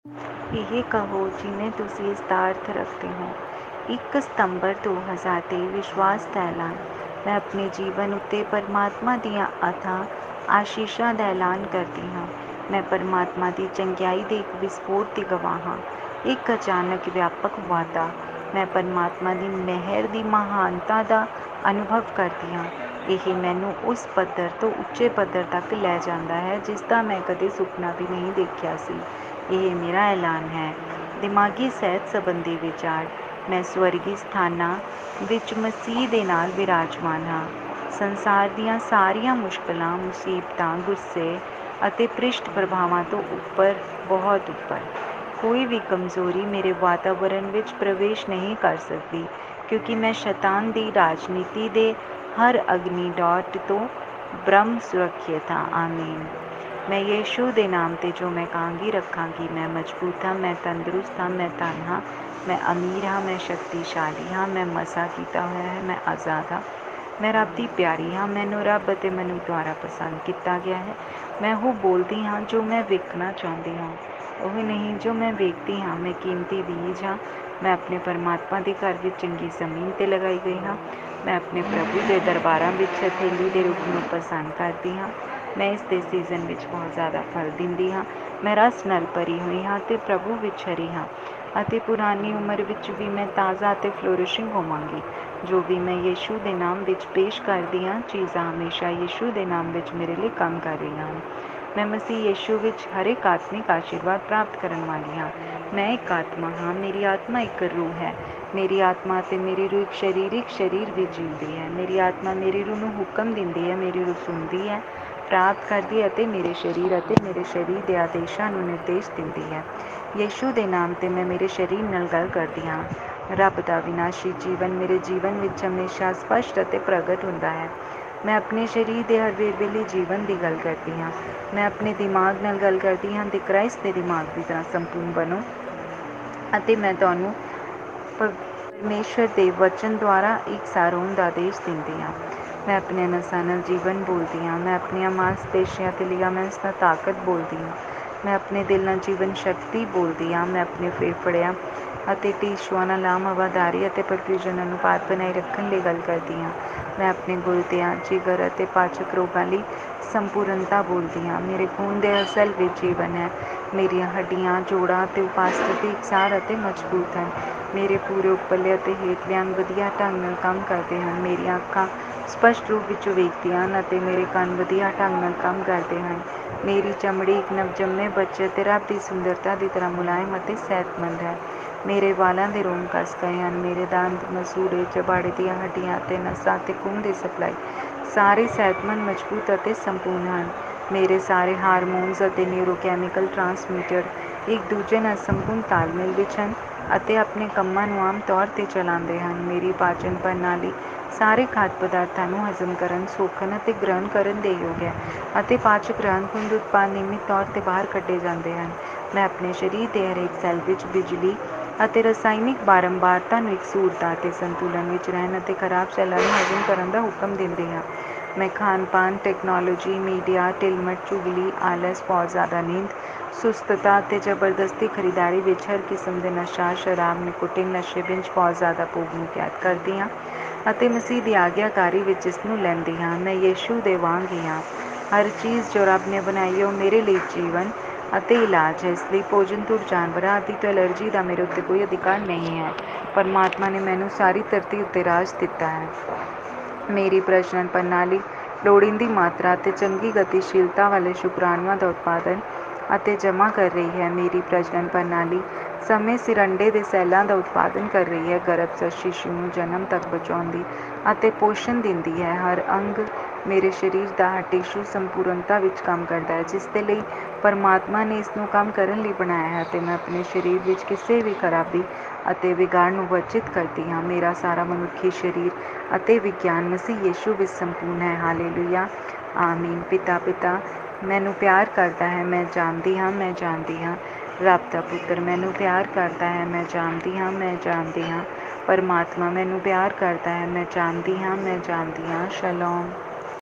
1st September 2023 Say it Like you Mean it (Audio in Punjabi)
ਇਹ ਕਹੋ ਜਿਵੇਂ ਤੁਸੀਂ ਇਸਦਾ ਅਰਥ ਰੱਖਦੇ ਹੋ
 1 ਸਤੰਬਰ 2023

 ਵਿਸ਼ਵਾਸ ਦੇ ਐਲਾਨ

 ਮੈਂ ਆਪਣੇ ਜੀਵਨ ਉੱਤੇ ਪ੍ਰਮਾਤਮਾ ਦੀਆਂ ਅਥਾਹ ਅਸੀਸਾਂ ਦਾ ਐਲਾਨ ਕਰਦੀ ਹਾਂ।  ਮੈਂ ਪ੍ਰਮਾਤਮਾ ਦੀ ਚੰਗਿਆਈ ਦੇ ਇੱਕ ਵਿਸਫੋਟ ਦੀ ਗਵਾਹ ਹਾਂ, ਇੱਕ ਅਚਾਨਕ ਵਿਆਪਕ ਵਾਧਾ।  ਮੈਂ ਪ੍ਰਮਾਤਮਾ ਦੀ ਮਿਹਰ ਦੀ ਉੱਤਮ ਮਹਾਨਤਾ ਦਾ ਅਨੁਭਵ ਕਰਦੀ ਹਾਂ। ਇਹ ਮੈਨੂੰ ਉਸ ਪੱਧਰ ਤੋਂ ਉੱਚੇ ਪੱਧਰ 'ਤੇ ਲੈ ਜਾਂਦਾ ਹੈ ਜਿਸਦਾ ਮੈਂ ਕਦੇ ਸੁਪਨਾ ਵੀ ਨਹੀਂ ਦੇਖਿਆ ਸੀ।  ਇਹ ਮੇਰਾ ਐਲਾਨ ਹੈ।

 ਦਿਮਾਗੀ ਸਿਹਤ ਸੰਬੰਧੀ ਵਿਚਾਰ

 ਮੈਂ ਸਵਰਗੀ ਸਥਾਨਾਂ ਵਿੱਚ ਮਸੀਹ ਦੇ ਨਾਲ ਬਿਰਾਜਮਾਨ ਹਾਂ, ਸੰਸਾਰ ਦੀਆਂ ਸਾਰੀਆਂ ਮੁਸ਼ਕਲਾਂ, ਮੁਸੀਬਤਾਂ, ਗੁੱਸੇ ਅਤੇ ਭ੍ਰਿਸ਼ਟ ਪ੍ਰਭਾਵਾਂ ਤੋਂ ਬਹੁਤ ਉੱਪਰ। ਕੋਈ ਵੀ ਕਮਜ਼ੋਰੀ ਮੇਰੇ ਵਾਤਾਵਰਣ ਵਿੱਚ ਪ੍ਰਵੇਸ਼ ਨਹੀਂ ਕਰ ਸਕਦੀ, ਕਿਉਂਕਿ ਮੈਂ ਸ਼ੈਤਾਨ ਦੀ ਰਣਨੀਤੀ ਦੇ ਹਰ ਅਗਨੀ ਡਾਰਟ ਤੋਂ ਬ੍ਰਹਮ ਸੁਰੱਖਿਅਤ ਹਾਂ ਆਮੀਨ
ਮੈਂ ਯਿਸੂ ਦੇ ਨਾਮ ਤੇ, ਜੋ ਮੈਂ ਕਹਾਂਗੀ ,ਰੱਖਾਂਗੀ(ਜੌਨ14:13)
 ਮੈਂ ਮਜ਼ਬੂਤ ​​ਹਾਂ।(ਐਫ਼ 6:10)
 ਮੈਂ ਤੰਦਰੁਸਤ ਹਾਂ। (1 ਪਤ 2:24)
 ਮੈਂ ਧੰਨ ਹਾਂ।(ਅਫ਼ 1:3)
 ਮੈਂ ਅਮੀਰ ਹਾਂ (2 ਕੋਰ 8:9)
 ਮੈਂ ਸ਼ਕਤੀਸ਼ਾਲੀ ਹਾਂ। (ਇਸ 40:29)
 ਮੈਂ ਮਸਹ ਕੀਤਾ ਹੋਇਆ ਹਾਂ (1 ਜੌਨ 2:20)
 ਮੈਂ ਆਜਾਦ ਹਾਂ।(ਯੂਹੰਨਾ 8:36)
 ਮੈਂ ਰੱਬ ਦੀ ਪਿਆਰੀ ਹਾਂ।(ਮਰਕੁਸ 1:11)
 ਮੈਨੂੰ ਰੱਬ ਅਤੇ ਮਨੁੱਖ ਦੁਆਰਾ ਪਸੰਦ ਕੀਤਾ ਗਿਆ ਹੈ।(ਉਤਪਤ 39:2-3)
 ਮੈਂ ਉਹ ਬੋਲਦੀ ਹਾਂ ਜੋ ਮੈਂ ਵੇਖਣਾ ਚਾਹੁੰਦੀ ਹਾਂ ਉਹ ਨਹੀਂ ਜੋ ਮੈਂ ਵੇਖਦੀ ਹਾਂ ।(2 ਕੁਰਿੰਥੀਆਂ 4:18)
 ਮੈਂ ਇੱਕ ਕੀਮਤੀ ਬੀਜ ਹਾਂ। (ਲੂਕਾ 8:15)
 ਮੈਂ ਆਪਣੇ ਪਰਮਾਤਮਾ ਦੇ ਘਰ ਵਿੱਚ ਚੰਗੀ ਜ਼ਮੀਨ 'ਤੇ ਲਗਾਈ ਗਈ ਹਾਂ। (ਜ਼ਬੂਰ 92:11)
 ਮੈਂ ਆਪਣੇ ਪ੍ਰਭੂ ਦੇ ਦਰਬਾਰਾਂ ਵਿੱਚ ਹਥੇਲੀ ਦੇ ਰੁੱਖ ਨੂੰ ਪਸੰਦ ਕਰਦੀ ਹਾਂ।(ਜ਼ਬੂਰ 92:12)
 ਮੈਂ ਇਸ ਦੇ ਸੀਜ਼ਨ ਵਿੱਚ ਬਹੁਤ ਜ਼ਿਆਦਾ ਫਲ ਦਿੰਦੀ ਹਾਂ।(ਜ਼ਬੂਰ 1:3)
 ਮੈਂ ਰਸ ਨਾਲ ਭਰੀ ਹੋਈ ਹੈ ਅਤੇ ਪ੍ਰਭੂ ਵਿੱਚ  ਹਰੀ ਹਾਂ(ਜ਼ਬੂਰ 92:14)
 ਅਤੇ ਪੁਰਾਣੀ ਉਮਰ ਵਿੱਚ ਵੀ ਮੈਂ ਤਾਜ਼ਾ ਅਤੇ ਫਲੋਰਿਸ਼ਿੰਗ ਹੋਵੇਗੀ ।(ਜ਼ਬੂਰ 92:14)
 ਜੋ ਵੀ ਮੈਂ ਯਿਸੂ ਦੇ ਨਾਮ ਵਿੱਚ ਪੇਸ਼ ਕਰਦੀ ਹਾਂ।(ਜ਼ਬੂਰ1:3)
 ਚੀਜ਼ਾਂ ਹਮੇਸ਼ਾਂ ਯਿਸੂ ਦੇ ਨਾਮ ਵਿੱਚ ਮੇਰੇ ਲਈ ਕੰਮ ਕਰ ਰਹੀਆਂ ਹਨ(ਰੋਮ 8:28)
 ਮੈਂ ਮਸੀਹ ਯਿਸੂ ਵਿੱਚ ਹਰ ਇੱਕ ਆਤਮਿਕ ਆਸ਼ੀਰਵਾਦ ਪ੍ਰਾਪਤ ਕਰਨ ਵਾਲੀ ਹਾਂ।(ਅਫ਼ 1:3)

 ਮੈਂ ਇੱਕ ਆਤਮਾ ਹਾਂ
 ਮੇਰੀ ਆਤਮਾ ਇੱਕ ਰੂਹ ਹੈ
 ਮੇਰੀ ਆਤਮਾ ਅਤੇ ਮੇਰੀ ਰੂਹ ਇੱਕ ਸਰੀਰਕ ਸਰੀਰ ਵਿੱਚ ਜੀਉਂਦੀ ਹੈ
 ਮੇਰੀ ਆਤਮਾ ਮੇਰੀ ਰੂਹ ਨੂੰ ਹੁਕਮ ਦਿੰਦੀ ਹੈ।ਮੇਰੀ ਰੂਹ ਸੁਣਦੀ ਹੈ, ਪ੍ਰਾਪਤ ਕਰਦੀ ਹੈ ਅਤੇ ਮੇਰੇ ਸਰੀਰ ਅਤੇ ਮੇਰੇ ਸਰੀਰ ਦੇ ਆਦੇਸ਼ਾਂ ਨੂੰ ਨਿਰਦੇਸ਼ ਦਿੰਦੀ ਹੈ

 ਯਿਸੂ ਦੇ ਨਾਮ ਤੇ, ਮੈਂ ਮੇਰੇ ਸਰੀਰ ਨਾਲ ਗੱਲ ਕਰਦੀ ਹਾਂ
 ਰੱਬ ਦਾ ਅਵਿਨਾਸ਼ੀ ਜੀਵਨ ਮੇਰੇ ਜੀਵਨ ਵਿੱਚ ਹਮੇਸ਼ਾਂ ਸਪਸ਼ਟ ਅਤੇ ਪ੍ਰਗਟ ਹੁੰਦਾ ਹੈ
 ✝ਮੈਂ ਆਪਣੇ ਸਰੀਰ ਦੇ ਹਰ ਵੇਰਵੇ ਲਈ ਜੀਵਨ ਦੀ ਗੱਲ ਕਰਦੀ ਹਾਂ।
✝ਮੈਂ ਆਪਣੇ ਦਿਮਾਗ ਨਾਲ ਗੱਲ ਕਰਦੀ ਹਾਂ ।ਦਿ ਕਰਾਇਸਟ ਦੇ ਦਿਮਾਗ ਦੀ ਤਰ੍ਹਾਂ ਸੰਪੂਰਨ ਬਣੋ ਅਤੇ ਮੈਂ ਤੁਹਾਨੂੰ ਪਰਮੇਸ਼ੁਰ ਦੇ ਬਚਨ ਦੁਆਰਾ  ਇਕਸਾਰ  ਹੋਣ ਦਾ ਆਦੇਸ਼ ਦਿੰਦੀ ਹਾਂ।
✝ਮੈਂ ਆਪਣੇ ਨਸਾਂ ਨਾਲ  ਜੀਵਨ ਬੋਲਦੀ ਹਾਂ।
✝ਮੈਂ ਆਪਣੀਆਂ ਮਾਸਪੇਸ਼ੀਆਂ ਅਤੇ ਲਿਗਾਮੈਂਟਸ ਨਾਲ ਤਾਕਤ ਬੋਲਦੀ ਹਾਂ।
✝ਮੈਂ ਆਪਣੇ ਦਿਲ ਨਾਲ  ਜੀਵਨਸ਼ਕਤੀ ਬੋਲਦੀ ਹਾਂ।
✝ਮੈਂ ਆਪਣੇ ਫੇਫੜਿਆਂ ਅਤੇ ਟਿਸ਼ੂਆਂ ਨਾਲ ਆਮ ਹਵਾਦਾਰੀ ਅਤੇ ਪਰਫਿusionਜ਼ਨ ਅਨੁਪਾਤ ਬਣਾਈ ਰੱਖਣ ਲਈ ਗੱਲ ਕਰਦੀ ਹਾਂ।
✝ਮੈਂ ਆਪਣੇ ਗੁਰਦਿਆਂ, ਜਿਗਰ ਅਤੇ ਪਾਚਕ ਰੋਗਾਂ ਲਈ  ਸੰਪੂਰਨਤਾ  ਬੋਲਦੀ ਹਾਂ।
✝ਮੇਰੇ ਖੂਨ ਦੇ ਹਰ ਸੈੱਲ ਵਿੱਚ ਜੀਵਨ ਹੈ ਮੇਰੀਆਂ ਹੱਡੀਆਂ, ਜੋੜਾਂ ਅਤੇ ਉਪਾਸਥੀ ,ਇਕਸਾਰ, ​​ਅਤੇ ਮਜ਼ਬੂਤ ​​ਹਨ ।
✝ਮੇਰੇ ਪੂਰੇ ਉਪਰਲੇ ਅਤੇ ਹੇਠਲੇ ਅੰਗ ਵਧੀਆ ਢੰਗ ਨਾਲ ਕੰਮ ਕਰਦੇ ਹਨ ।
✝ਮੇਰੀਆਂ ਅੱਖਾਂ ਸਪਸ਼ਟ ਰੂਪ ਵਿੱਚ ਵੇਖਦੀਆਂ ਹਨ  ਅਤੇ
✝ਮੇਰੇ ਕੰਨ ਵਧੀਆ *ੰਗ  ਨਾਲ ਕੰਮ ਕਰਦੇ ਹਨ । ਮੇਰੀ ਚਮੜੀ ਇੱਕ ਨਵਜੰਮੇ ਬੱਚੇ ਅਤੇ ਰੱਬ ਦੀ ਸੁੰਦਰਤਾ ਦੀ ਤਰ੍ਹਾਂ  ਮੁਲਾਇਮ ਅਤੇ ਸਿਹਤਮੰਦ ਹੈ।
✝ਮੇਰੇ ਵਾਲਾਂ ਦੇ ਰੋਮ ਕੱਸ ਗਏ ਹਨ।
✝ਮੇਰੇ ਦੰਦ, ਮਸੂੜੇ, ਜਬਾੜੇ ਦੀਆਂ ਹੱਡੀਆਂ ਅਤੇ ਨਸਾਂ ਅਤੇ ਖੂਨ ਦੀ ਸਪਲਾਈ ਸਾਰੇ ਸਿਹਤਮੰਦ, ਮਜ਼ਬੂਤ ​​ਅਤੇ ਸੰਪੂਰਨ ਹਨ
✝ਮੇਰੇ ਸਾਰੇ ਹਾਰਮੋਨਸ ਅਤੇ ਨਿਰੋਕੈਮੀਕਲ ਟ੍ਰਾਂਸਮੀਟਰ ਇੱਕ ਦੂਜੇ ਨਾਲ ਸੰਪੂਰਨ ਤਾਲਮੇਲ ਵਿੱਚ ਹਨ ਅਤੇ ਆਪਣੇ ਕੰਮਾਂ ਨੂੰ ਆਮ ਤੌਰ ਤੇ ਚਲਾਉਂਦੇ ਹਨ ।
✝ਮੇਰੀ ਪਾਚਨ ਪ੍ਰਣਾਲੀ ਸਾਰੇ ਖਾਧ ਪਦਾਰਥਾਂ ਨੂੰ ਹਜ਼ਮ ਕਰਨ, ਸੋਖਣ ਅਤੇ ਗ੍ਰਹਿਣ ਕਰਨ ਦੇ ਯੋਗ ਹੈ ਅਤੇ ਪਾਚਕ ਰਹਿੰਦ -ਖੂੰਹਦ ਉਤਪਾਦ ਨਿਯਮਤ ਤੌਰ ਤੇ ਬਾਹਰ ਕੱੇ ਜਾਂਦੇ ਹਨ ।
✝ ਮੈਂ ਆਪਣੇ ਸਰੀਰ ਦੇ ਹਰੇਕ ਸੈੱਲ ਵਿੱਚ ਬਿਜਲੀ ਅਤੇ ਰਸਾਇਣਕ ਬਾਰੰਬਾਰਤਾ ਨੂੰ ਇਕਸੁਰਤਾ ਅਤੇ ਸੰਤੁਲਨ ਵਿੱਚ ਰਹਿਣ ਅਤੇ ਖਰਾਬ ਸੈੱਲਾਂ ਨੂੰ ਹਜ਼ਮ ਕਰਨ ਦਾ ਹੁਕਮ ਦਿੰਦੀ ਹਾਂ।
✝ਮੈਂ ਖਾਣ-ਪੀਣ, ਤਕਨਾਲੋਜੀ, ਮੀਡੀਆ, ਢਿੱਲ-ਮੱਠ, ਚੁਗਲੀ, ਆਲਸ, ਬਹੁਤ ਜ਼ਿਆਦਾ ਨੀਂਦ, ਸੁਸਤਤਾ, ਅਤੇ ਜਬਰਦਸਤੀ ਖਰੀਦਦਾਰੀ ਵਿੱਚ ਹਰ ਕਿਸਮ ਦੇ ਨਸ਼ਾ (ਸ਼ਰਾਬ, ਨਿਕੋਟੀਨ, ਨਸ਼ੇ) ਬਿੰਜ (ਬਹੁਤ ਜ਼ਿਆਦਾ ਭੋਗ) ਨੂੰ ਕੈਦ ਕਰਦੀ ਹਾਂ ਅਤੇ ਮਸੀਹ ਦੀ ਆਗਿਆਕਾਰੀ ਵਿੱਚ ਇਸਨੂੰ ਲੈਂਦੀ ਹਾਂ। ਮੈਂ ਯਿਸੂ ਵਾਂਗ ਹੀ ਹਾਂ।
✝ਹਰ ਚੀਜ਼ ਜੋ ਰੱਬ ਨੇ ਬਣਾਈ ਹੈ ਉਹ ਮੇਰੇ ਲਈ ਜੀਵਨ ਅਤੇ ਇਲਾਜ ਹੈ, ਇਸਲਈ ਭੋਜਨ, ਧੂੜ, ਜਾਨਵਰਾਂ ਆਦਿ ਤੋਂ ਐਲਰਜੀ ਦਾ ਮੇਰੇ ਉੱਤੇ ਕੋਈ ਅਧਿਕਾਰ ਨਹੀਂ ਹੈ। ਪਰਮਾਤਮਾ ਨੇ ਮੈਨੂੰ ਸਾਰੀ ਧਰਤੀ ਉੱਤੇ ਰਾਜ ਦਿੱਤਾ ਹੈ
✝ 🙎🏻‍♂ ♂ਮੇਰੀ ਪ੍ਰਜਨਨ ਪ੍ਰਣਾਲੀ ਲੋੜੀਂਦੀ ਮਾਤਰਾ ਅਤੇ ਚੰਗੀ ਗਤੀਸ਼ੀਲਤਾ ਵਾਲੇ ਸ਼ੁਕਰਾਣੂਆਂ ਦਾ ਉਤਪਾਦਨ ਅਤੇ ਜਮ੍ਹਾਂ ਕਰ ਰਹੀ ਹੈ
✝ 🙍🏻‍♀ ‍♀ਮੇਰੀ ਪ੍ਰਜਨਨ ਪ੍ਰਣਾਲੀ ਸਮੇਂ ਸਿਰ ਅੰਡੇ ਦੇ ਸੈੱਲਾਂ ਦਾ ਉਤਪਾਦਨ ਕਰ ਰਹੀ ਹੈ, ਗਰੱਭਸਥ ਸ਼ੀਸ਼ੂ ਨੂੰ ਜਨਮ  ਤੱਕ ਬਚਾਉਂਦੀ ਅਤੇ ਪੋਸ਼ਣ ਦਿੰਦੀ ਹੈ ।
✝  ਹਰ ਅੰਗ, ਮੇਰੇ ਸਰੀਰ ਦਾ ਹਰ ਟਿਸ਼ੂ ਸੰਪੂਰਨਤਾ ਵਿੱਚ ਕੰਮ ਕਰਦਾ ਹੈ । ਜਿਸਦੇ ਲਈ ਪਰਮਾਤਮਾ ਨੇ ਇਸਨੂੰ ਕੰਮ ਕਰਨ ਲਈ ਬਣਾਇਆ ਹੈ, ਅਤੇ ਮੈਂ ਆਪਣੇ ਸਰੀਰ ਵਿੱਚ ਕਿਸੇ ਵੀ ਖਰਾਬੀ ਅਤੇ ਵਿਗਾੜ ਨੂੰ ਵਰਜਿਤ ਕਰਦੀ ਹਾਂ।
✝  ਮੇਰਾ ਸਾਰਾ ਮਨੁੱਖੀ ਸਰੀਰ ਅਤੇ ਵਿਗਿਆਨ ਮਸੀਹ ਯਿਸੂ ਵਿੱਚ ਸੰਪੂਰਨ ਹੈ।ਹਲਲੂਯਾਹ! ਆਮੀਨ

 ਪਿਤਾ ਪਿਤਾ, ਮੈਨੂੰ ਪਿਆਰ ਕਰਦਾ ਹੈ, ਮੈਂ  ਜਾਣਦੀ ਹਾਂ ਮੈਂ ਜਾਣਦੀ ਹਾਂ
 ਰੱਬ ਦਾ ਪੁੱਤਰ, ਮੈਨੂੰ ਪਿਆਰ ਕਰਦਾ ਹੈ, ਮੈਂ ਜਾਣਦੀ ਹਾਂ ਮੈਂ ਜਾਣਦੀ ਹਾਂ
 ਪ੍ਰਮਾਤਮਾ ਮੈਨੂੰ ਪਿਆਰ ਕਰਦਾ ਹੈ, ਮੈਂ  ਜਾਣਦੀ ਹਾਂ ਮੈਂ ਜਾਣਦੀ ਹਾਂ

 ਸ਼ਲੋਮ